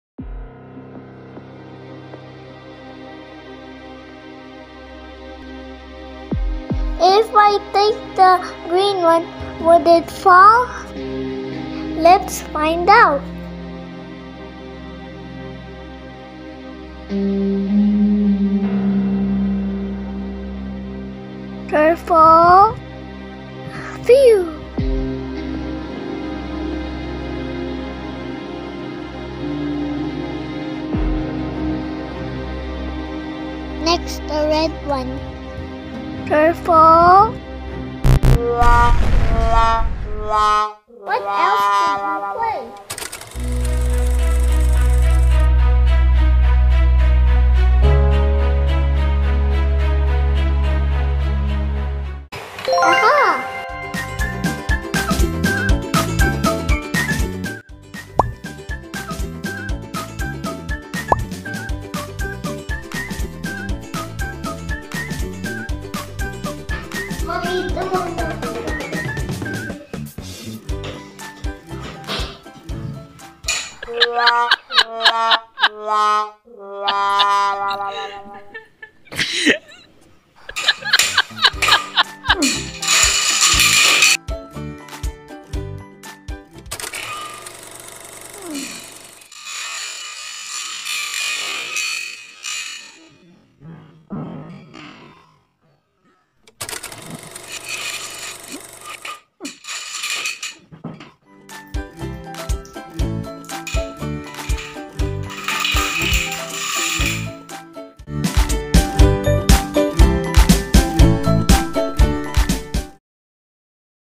🎴 UNO DOMINO! | AXL ROCKS! 🎸
AXL thinks of other ways to play with the UNO STACKO. He thinks of UNO DOMINO! ENJOY!

𝐂𝐑𝐄𝐃𝐈𝐓𝐒:
Other music found here is on CapCut.